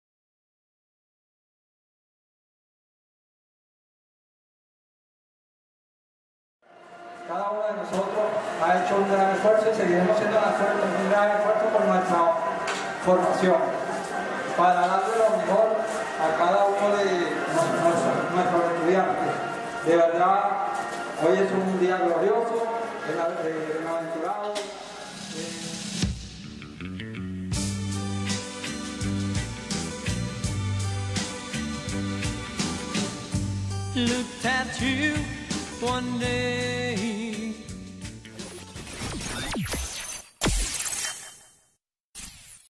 ASCENSO A DOCENTES UPTYAB
Luego de la debida experticia por parte del Ministerio del Poder Popular para la Educación Universitaria, Ciencia y Tecnología (Mppeuct), un grupo de educadores de la Universidad Politécnica Territorial de Yaracuy “Arístides Bastidas” (UPTYAB), reciben aprobación de ascenso a la Categoría Docente inmediata superior.